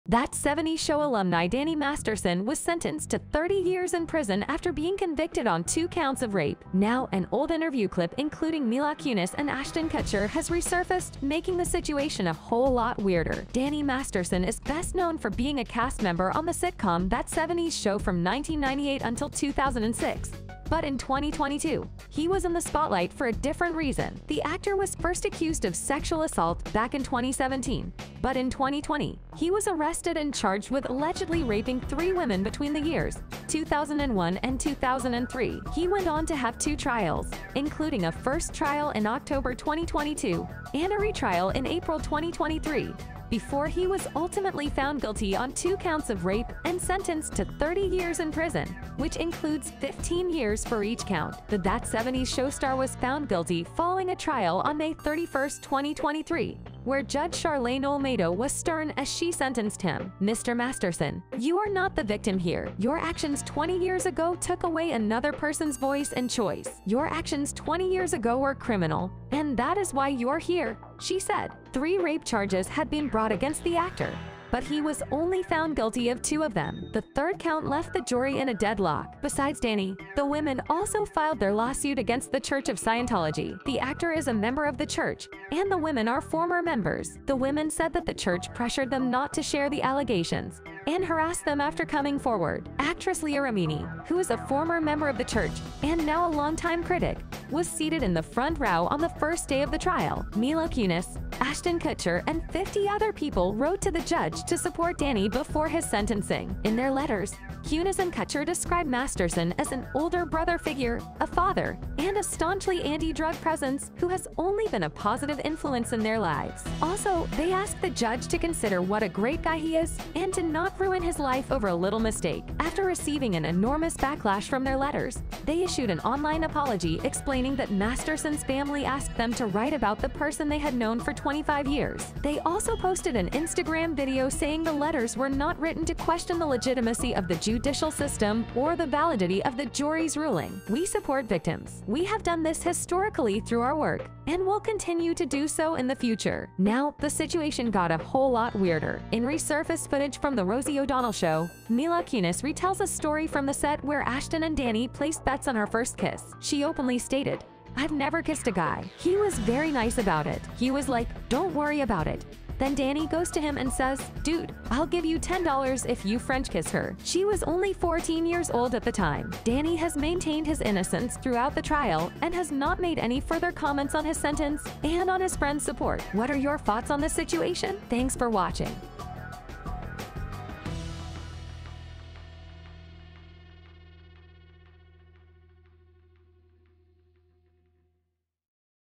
Danny Masterson: From Sitcom Greatness To 30 Years In Prison!
In a stunning turn of events, this video delves into the shocking trial of former "That '70s Show" star Danny Masterson, who was recently convicted and sentenced to 30 years in prison for two cases of rape. The trial sent shockwaves through Hollywood, and controversy ensued when fellow co-stars Ashton Kutcher and Mila Kunis penned letters to the judge advocating for leniency in Masterson's sentencing. Join us as we explore the details of this high-profile case, the court's decision, and the divisive response from Kutcher and Kunis, raising questions about the intersection of celebrity, justice, and accountability in the entertainment industry.